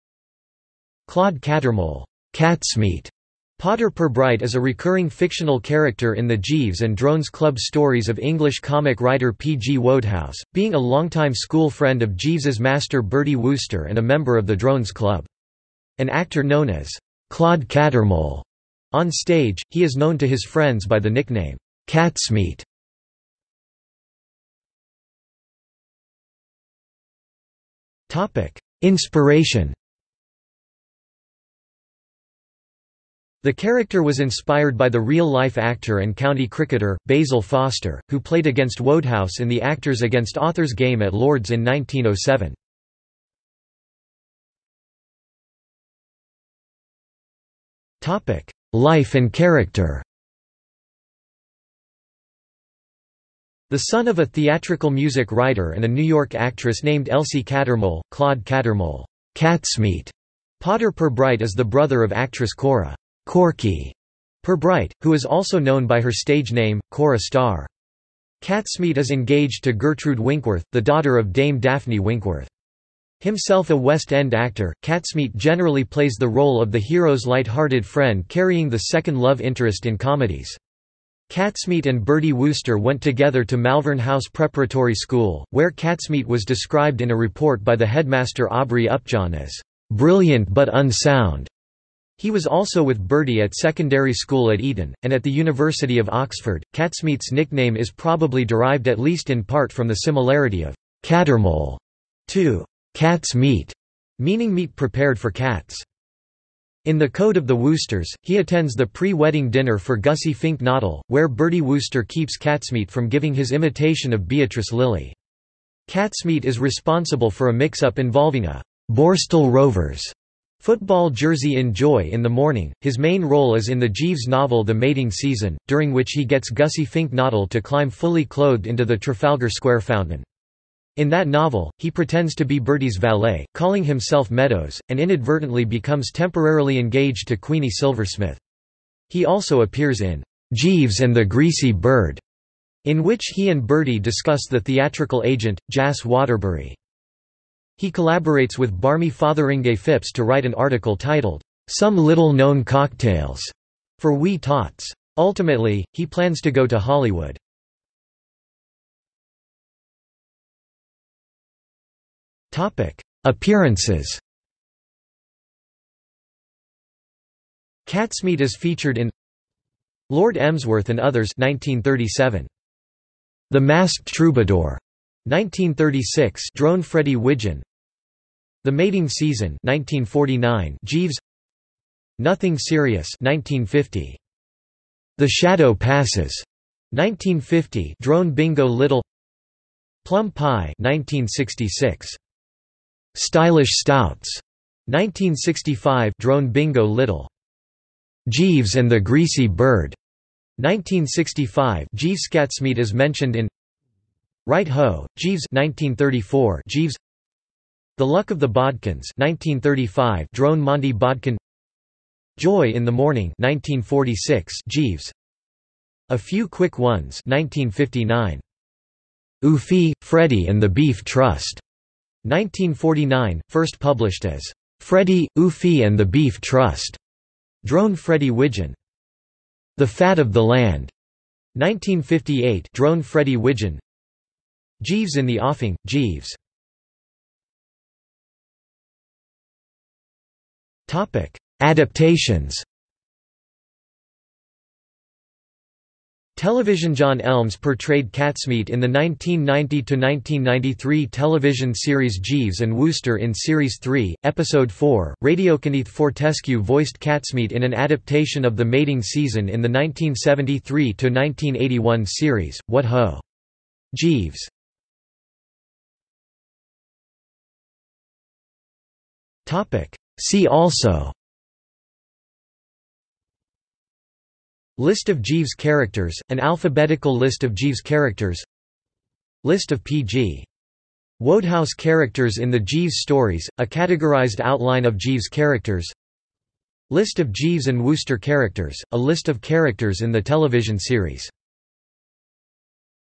Catsmeat Potter-Pirbright | Wikipedia audio article
This is an audio version of the Wikipedia Article:
Catsmeat Potter-Pirbright

00:00:24 1 Inspiration
00:00:44 2 Life and character
00:02:38 3 Appearances
00:03:57 4 Adaptations
00:04:25 5 See also

- Socrates

SUMMARY
Claude Cattermole "Catsmeat" Potter-Pirbright is a recurring fictional character in the Jeeves and Drones Club stories of English comic writer P. G. Wodehouse, being a longtime school friend of Jeeves's master Bertie Wooster and a member of the Drones Club. An actor known as "Claude Cattermole" on stage, he is known to his friends by the nickname "Catsmeat".